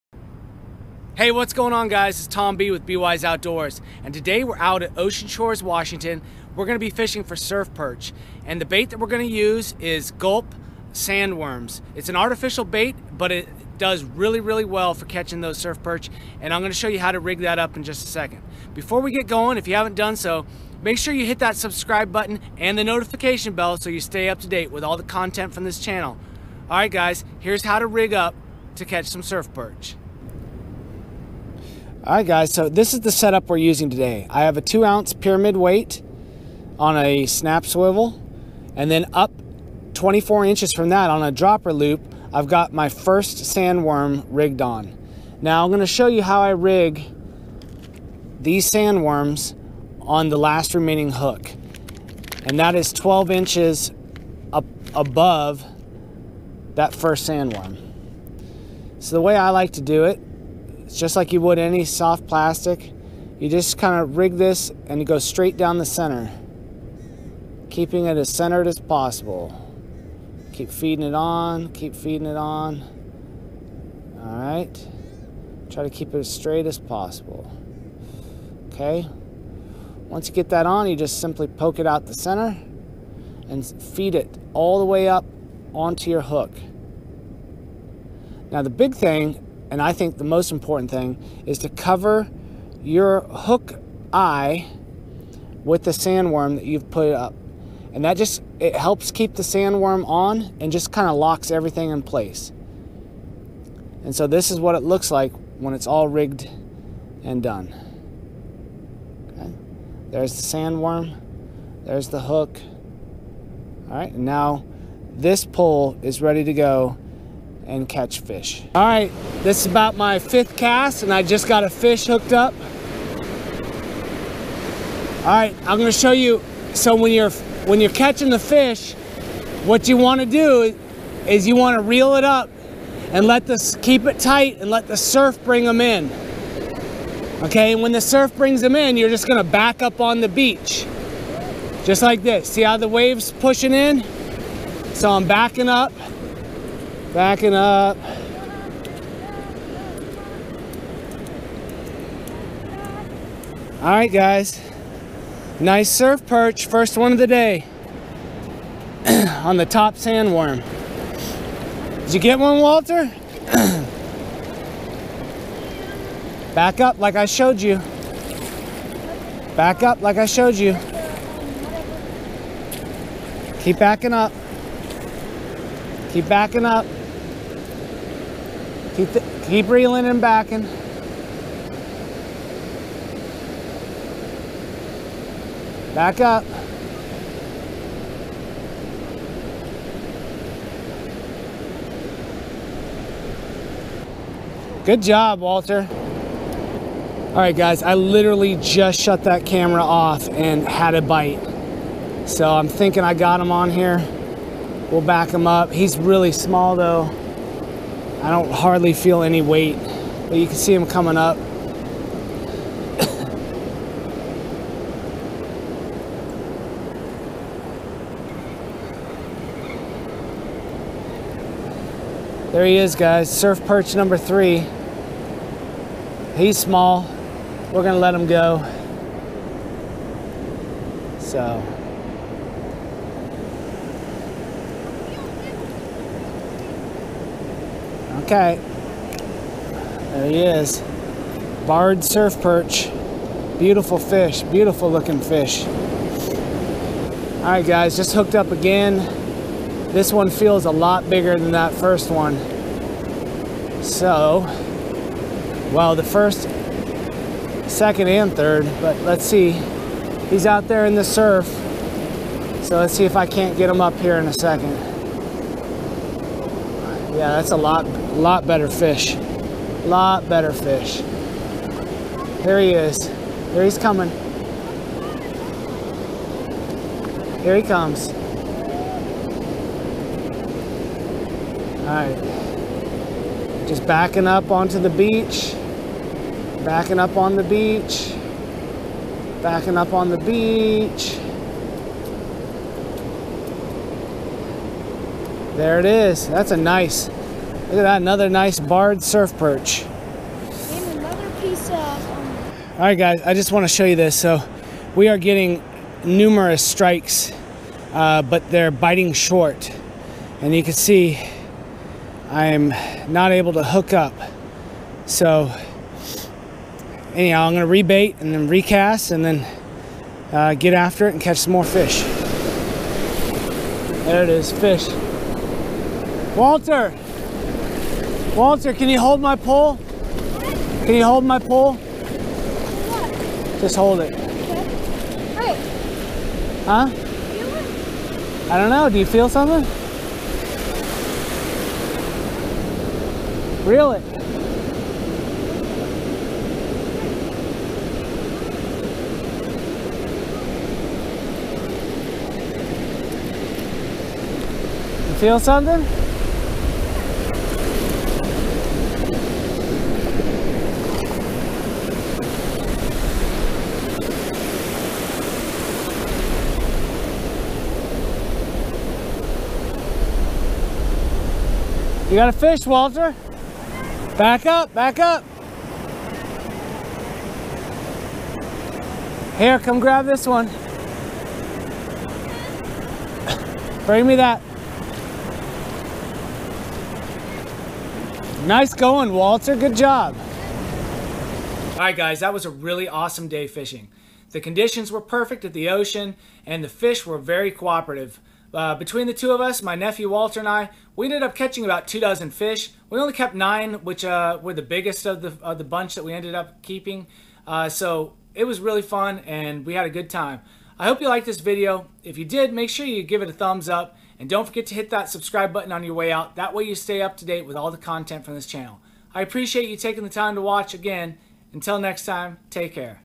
Washington Surf Perch Fishing
This video show tips and techniques on to setup and fish for surf perch.  In a few short hours my nephew Walter and I catch two dozen surf perch.